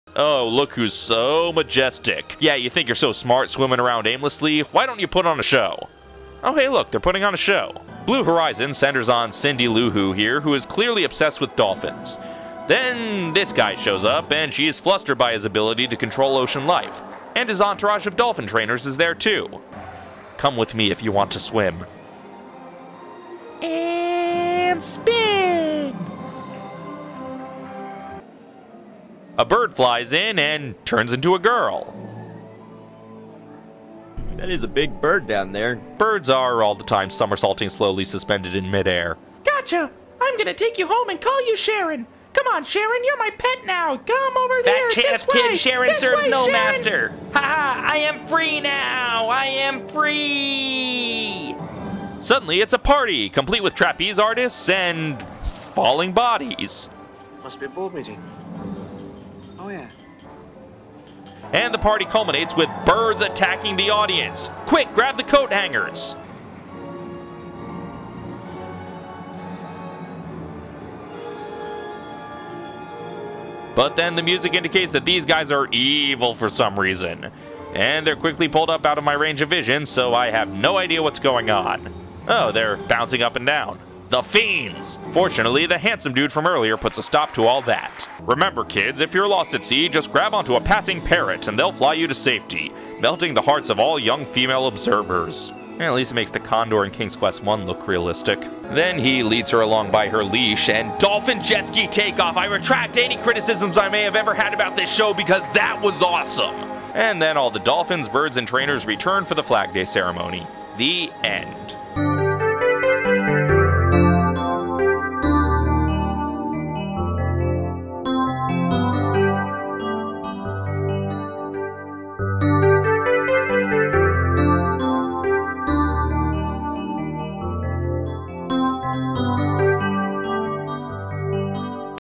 Dave Riffs SeaWorld's Blue Horizons - The Best of the Blitzstravaganza
DOLPHIN JETSKI TAKEOFF!

From Blitzstravaganza #12: Manta, Key West, and Blue Horizons at SeaWorld

I'm working hard on the Blitzstravafornia, and $2+ Patrons can get a few early looks now!